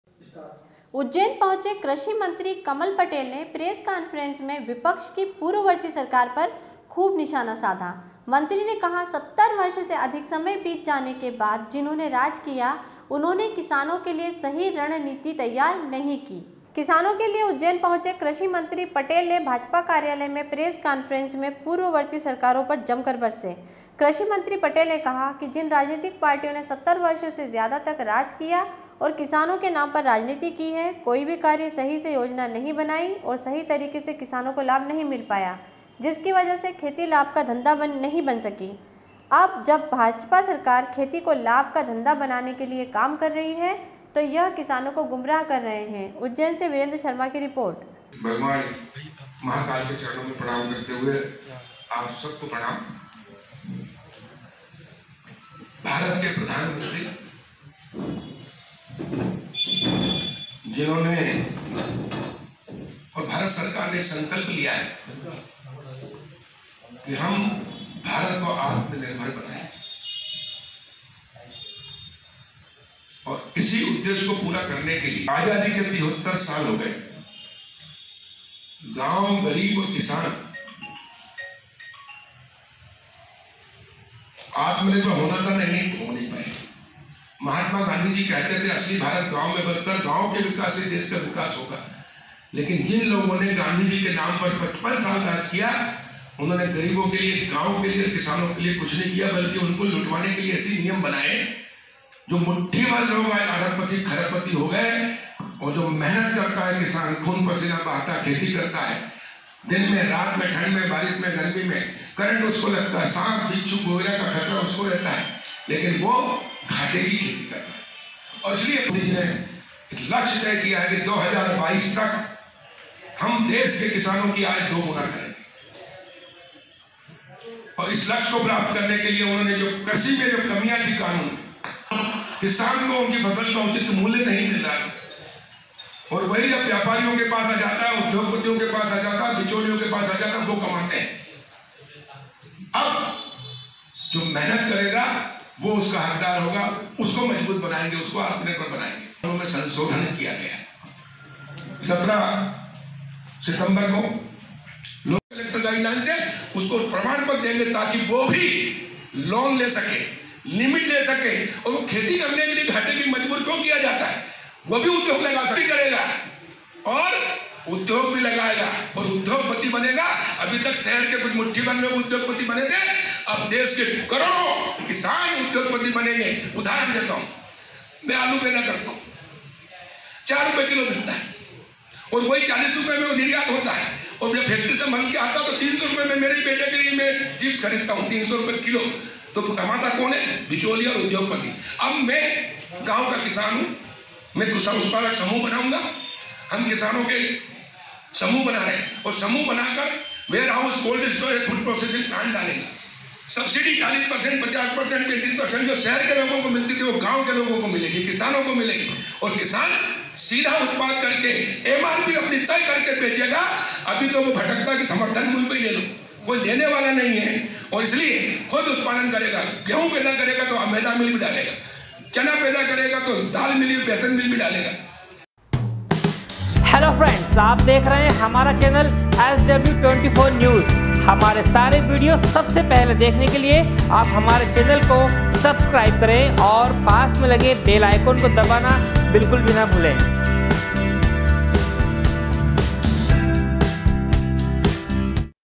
Ujjain- उज्जैन पहुंचे कृषि मंत्री कमल पटेल ने विपक्ष पूर्ववर्ती सरकार पर निशाना साधा
Ujjain- SW 24 News Madhyapradesh ujjain SW24news #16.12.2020
उज्जैन पहुंचे कृषि मंत्री कमल पटेल ने प्रेस कॉन्फ्रेंस में विपक्ष की पूर्ववर्ती सरकार पर खूब निशाना साधा। मंत्री ने कहा 70 वर्ष से अधिक समय बीत जाने के बाद जिन्होंने राज किया। उन्होंने किसानों के लिए सही रणनीति तैयार नहीं की। उज्जैन से वीरेंद्र शर्मा की रिपोर्ट।